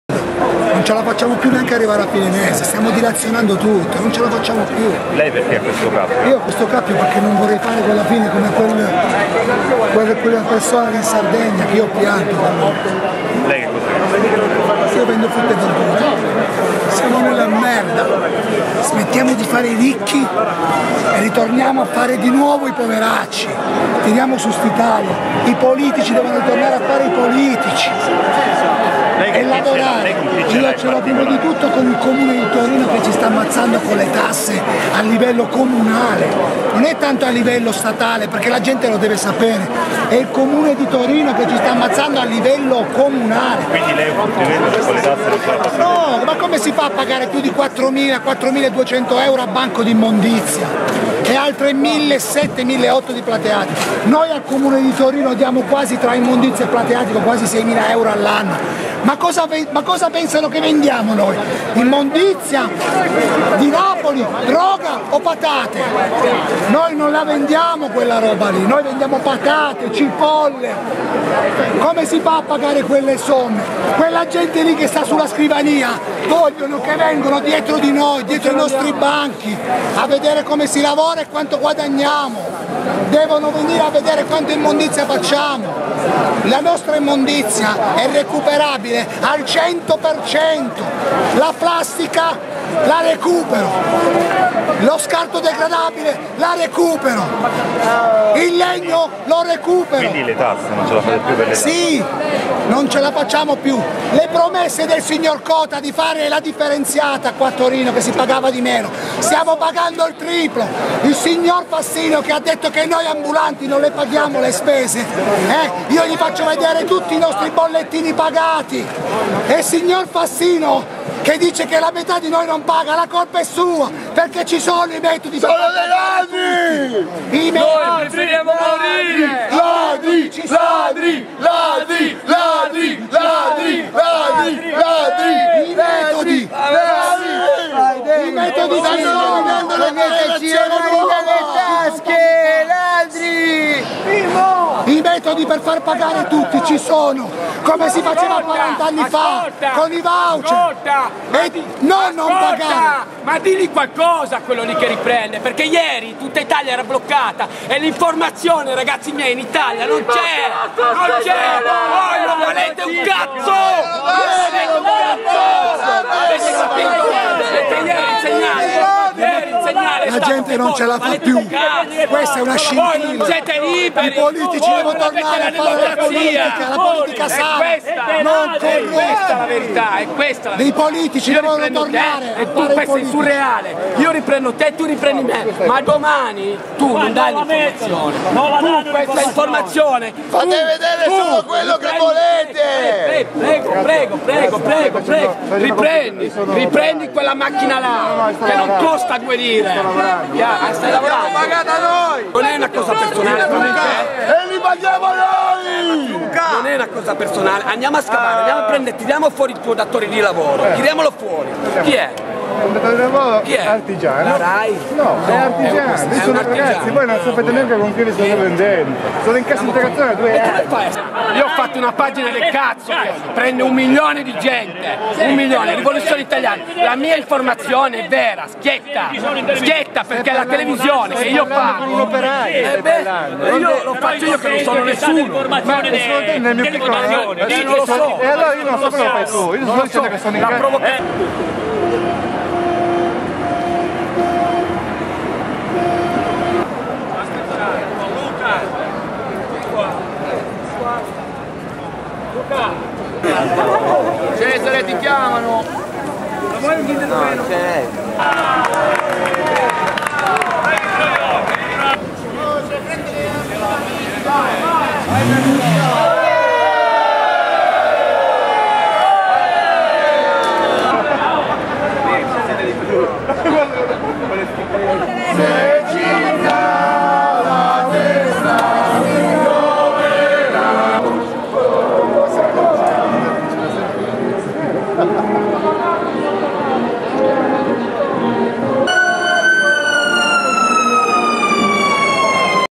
Protesta dei forconi: popolo disperato Torino
Protesta dei forconi: popolo disperato Torino 9-10 dicembre torino, Guardare le foto e video